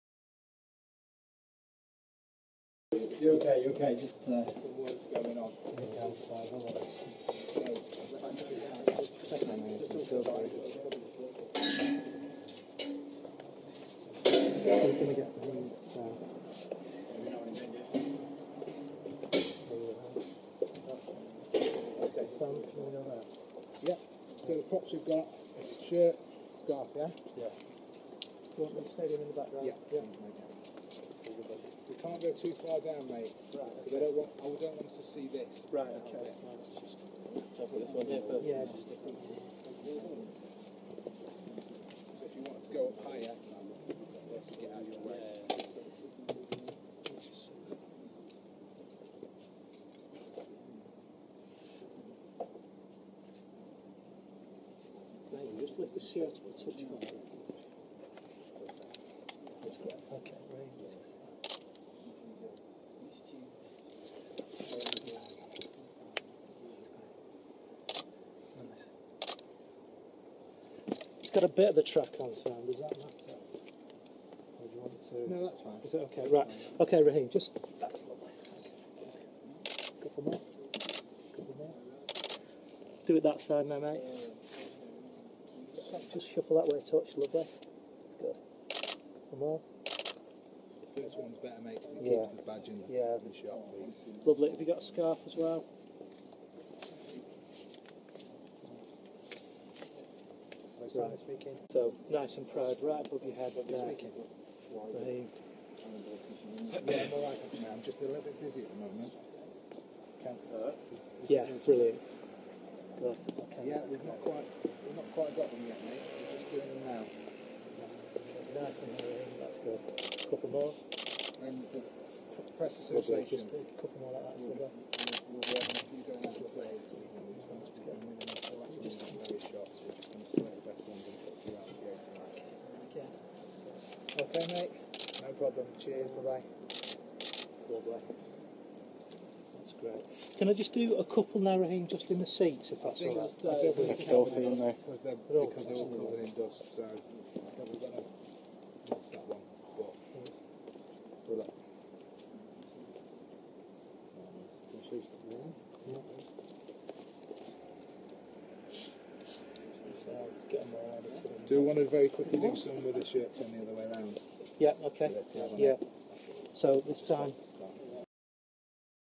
Raheem Sterling tours the Etihad Stadium
Taken from MCFC's periscope broadcast on the evening of the 14th July 2015